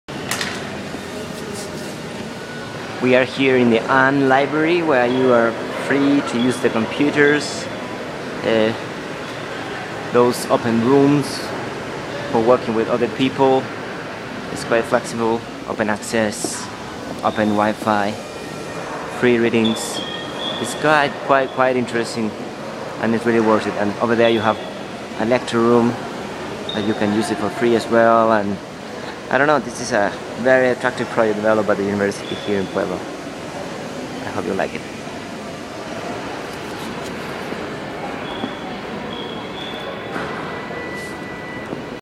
The unLibrary: no physical books but openness to infomration
Centro Cultural Universitario | Dirección General de Bibliotecas de la Benemérita Universidad Autónoma de Puebla (BUAP) , Puebla. México.